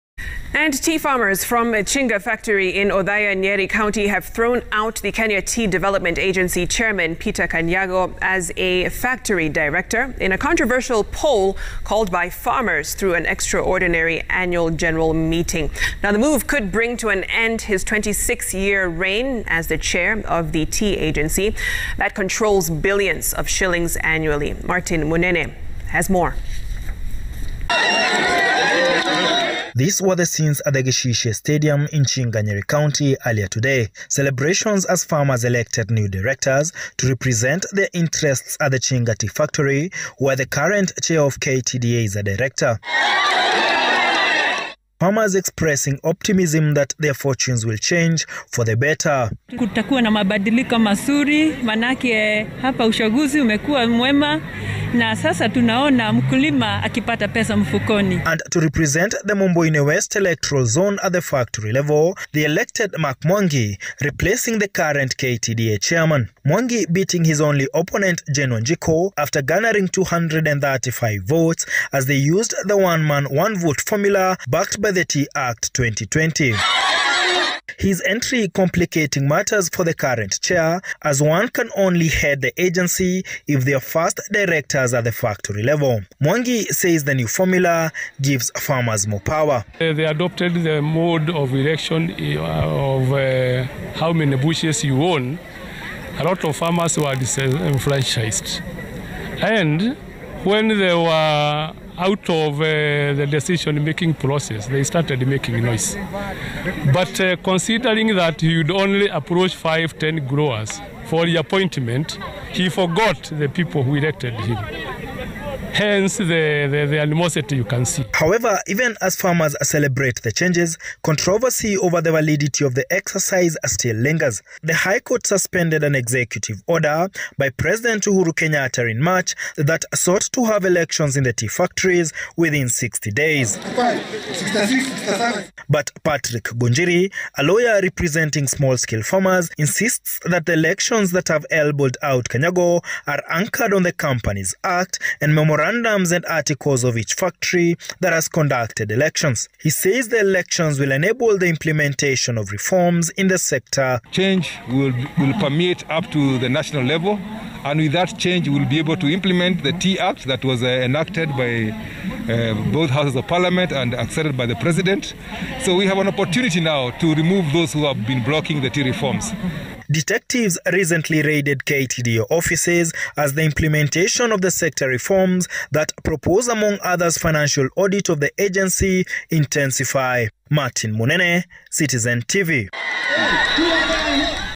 KTDA chairman Peter Kanyago removed as a director at Chinga Tea Factory
Tea farmers voted out Kanyago who has served as a director for 26 years
Kanyago has been replaced by Mark Mwangi who garnered 235 votes